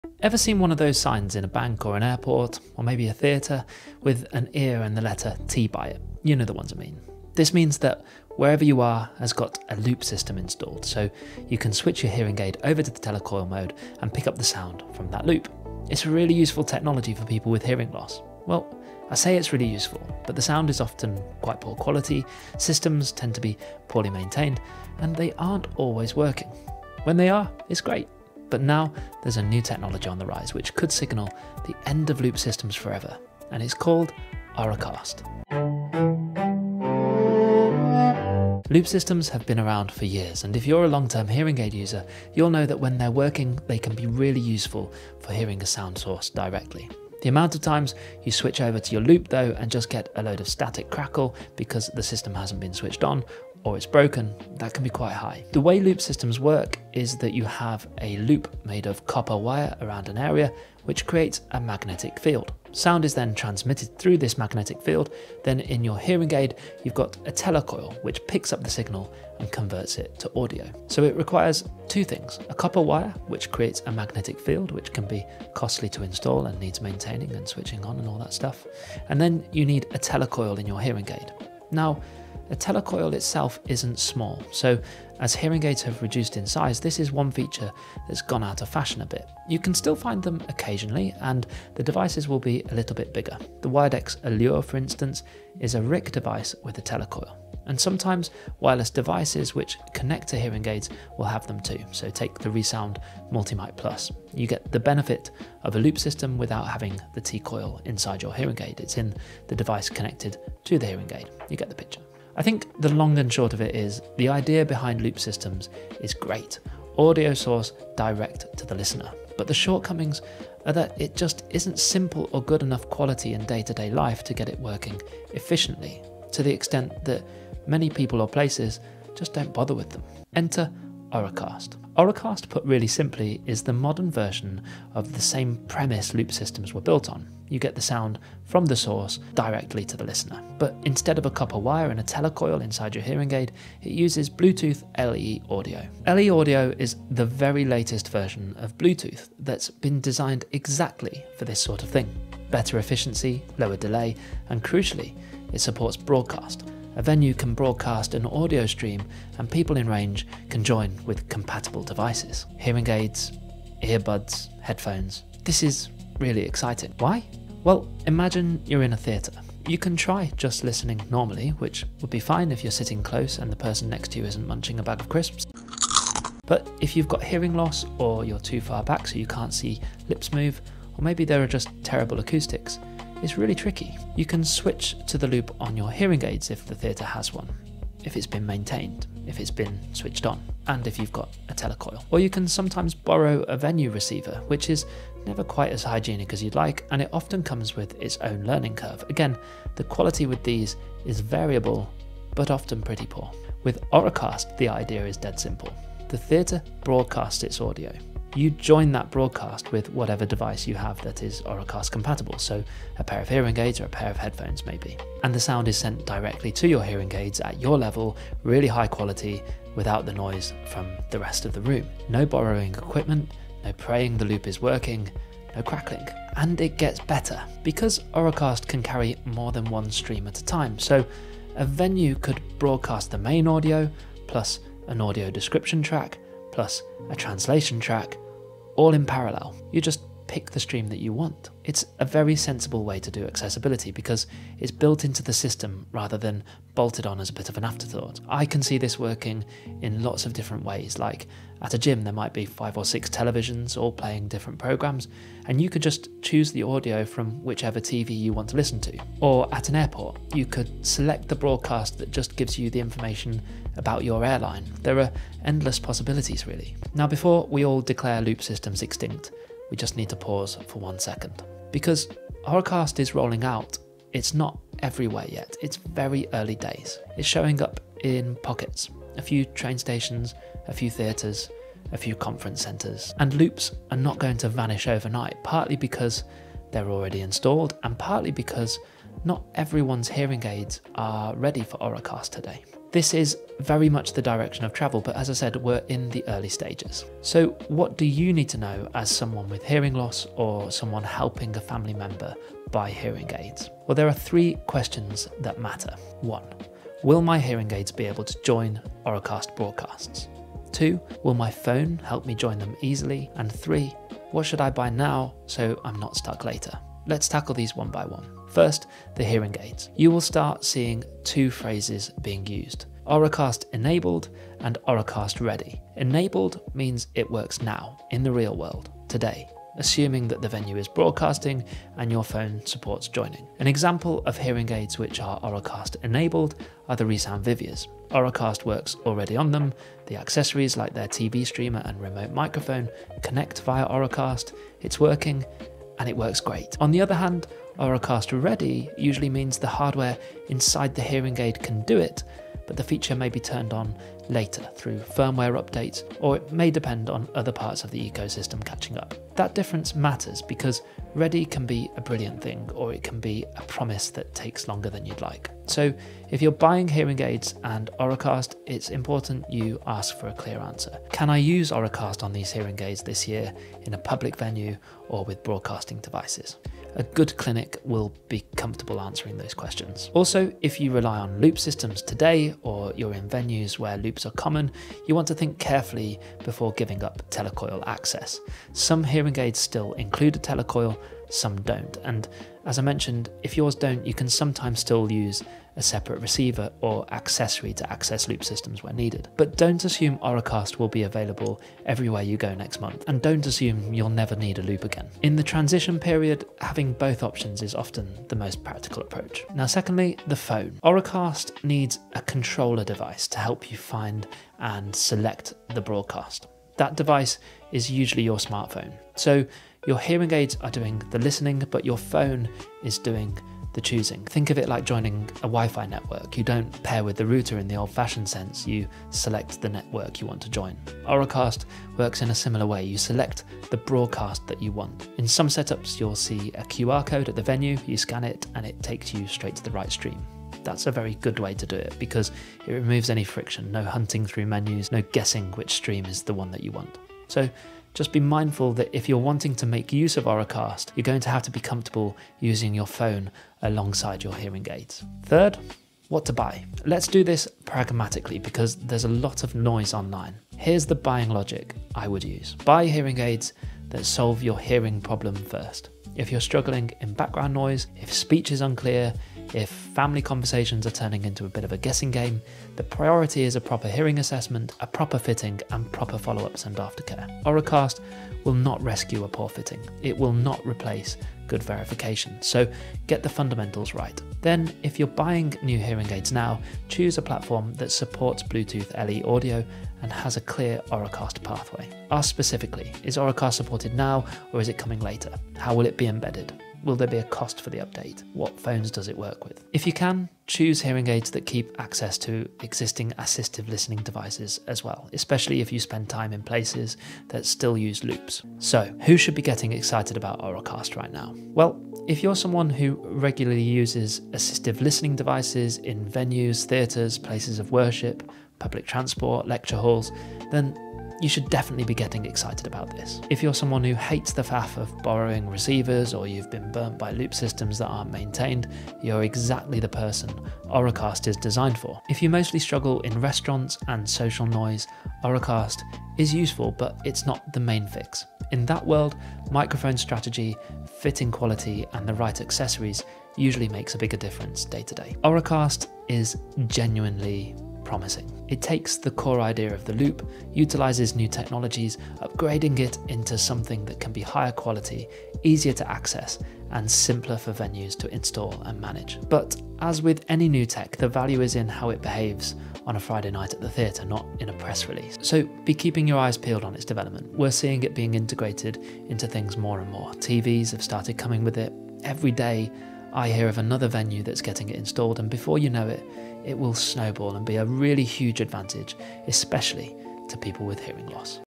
Auracast Explained: the Loop System Killer?
That ear-and-T sign is meant to help. In real life, loop systems can be brilliant… or completely useless.

Auracast is the first serious upgrade to the same idea: get the sound from the source straight to you, without fighting the room. It uses Bluetooth LE Audio and broadcast, so venues can transmit audio and compatible devices can join it.

In this video I cover:
 • why loops fail so often (even in good venues)
 • what Auracast actually is, in plain English
 • where it will matter most first (theatre, travel, public announcements)
 • “Auracast-enabled” vs “Auracast-ready” and why the wording matters
 • what to ask before you buy your next pair of hearing aids

Auracast is early. Loops are not disappearing overnight. The smart move is knowing what works today, what’s coming next, and how to avoid buying yourself into a dead end.

If you want help choosing the right long-term setup, explore Alto’s assessments and treatment plans: altohear.co.uk

00:38 How Loop Systems Work
02:10 What is Auracast
05:03 3 Questions You Need to Ask
07:34 What to Buy